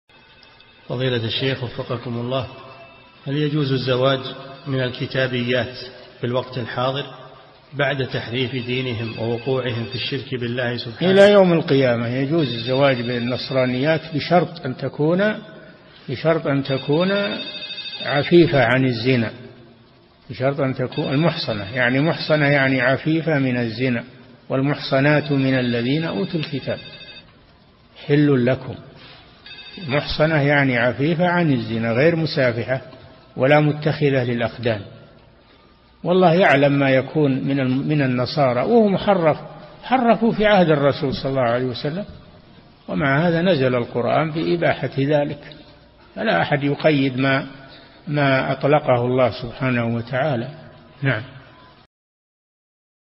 [499 -939] هل يجوز الزواج من الكتابيات في الوقت الحاضر بعد تحريف دينهم ووقوعهم في الشرك؟
-[499 -939] السؤال: هل يجوز الزواج من الكتابيات في الوقت الحاضر بعد تحريف دينهم ووقوعهم في الشرك بالله سبحانه؟
الجواب: إلى يوم القيامة يجوز الزواج بالنصرانيات بشرط أن تكون عفيفة عن الزنا بشرط أن تكون محصنة، المحصنة يعني عفيفة من الزنا {والمحصنات من الذين أوتوا الكتاب} حل لكم، محصنة يعني عفيفة عن الزنا غير مسافحة ولا متخذة للأخدان، والله يعلم ما يكون من النصارى وهم حرفوا في عهد الرسول ﷺ ومع هذا نزل القرآن بإباحة ذلك فلا أحد يقيد ما أطلقه الله سبحانه وتعالى.
للاشتراك في قناة الشيخ صالح الفوزان

للاستفسار والإقتراحات والدعم
رئيس الفريق
لمشروع كبار العلماء